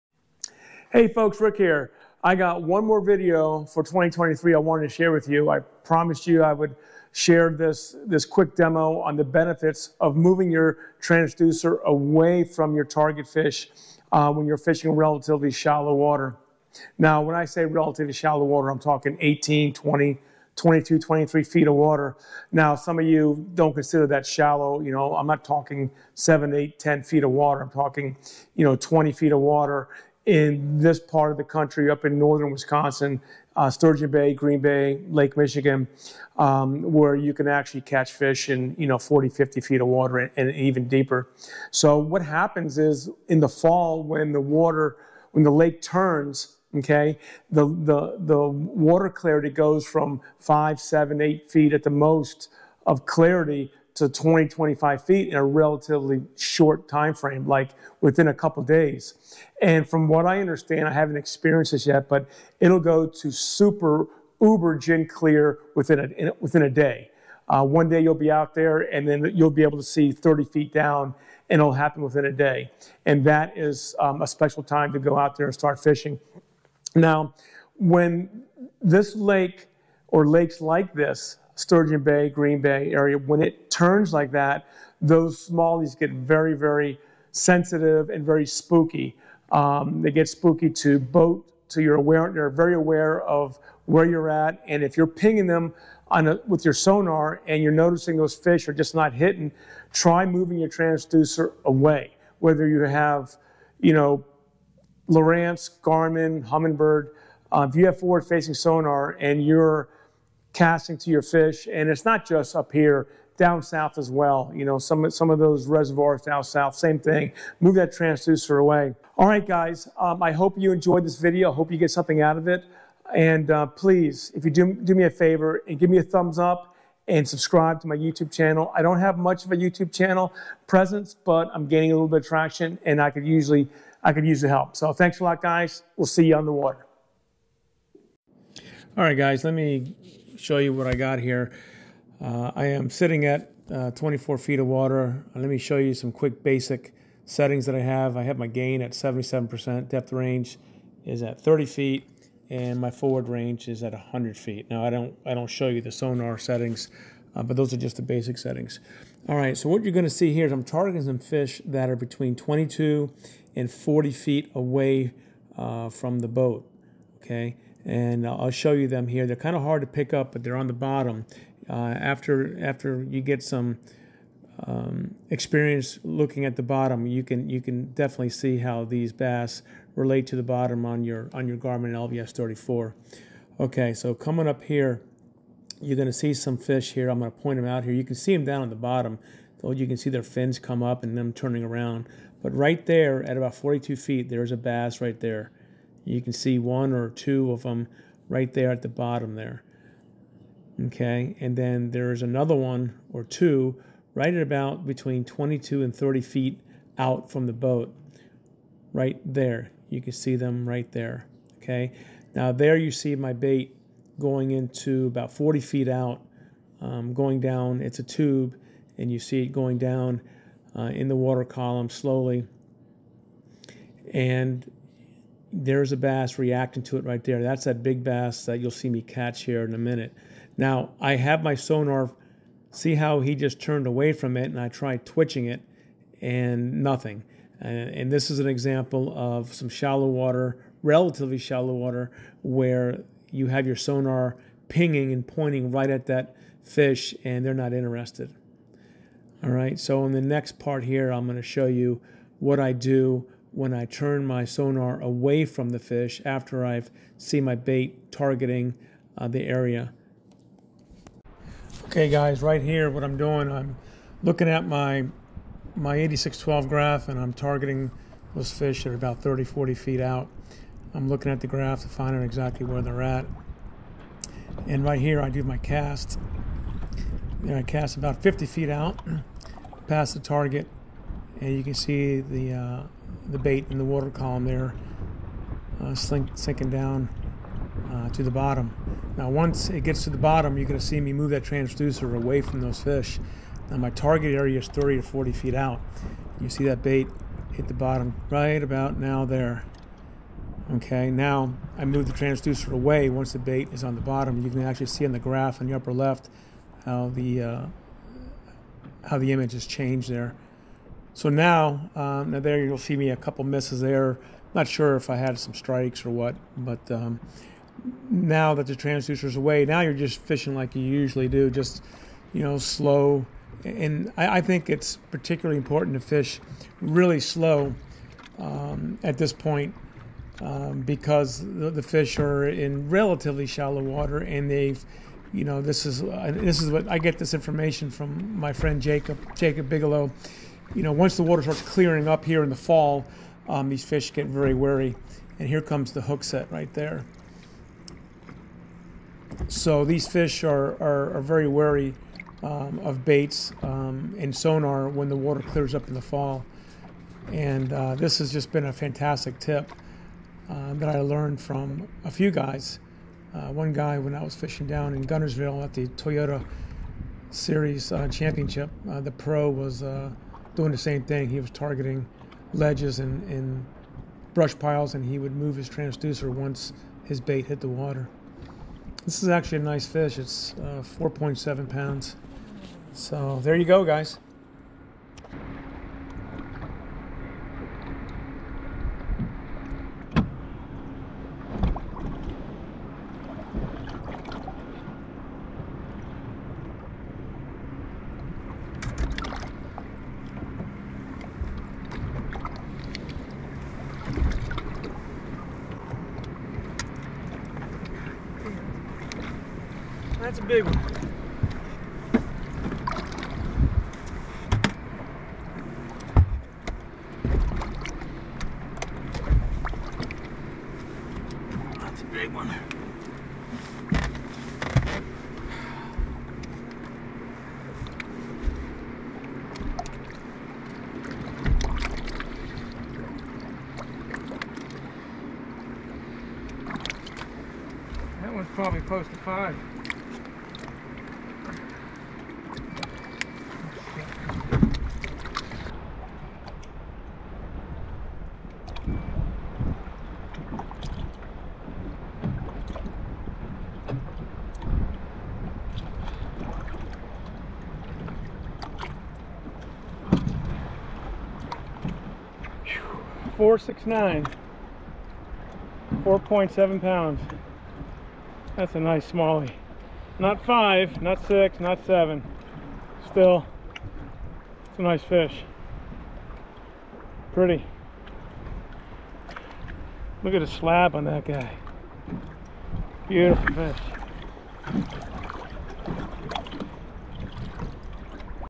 How to use Livescope on spooky bass
Smallmouth fishing on Green Bay - Part 2.  This is a demo on how to use your  Garmin Livescope and turn the transducer away from your target.  If you are seeing your targeted bass shy away from your bait, it may not necessarily be your bait selection.  Sometimes these fish are spooked by the constant pinging of your transducer.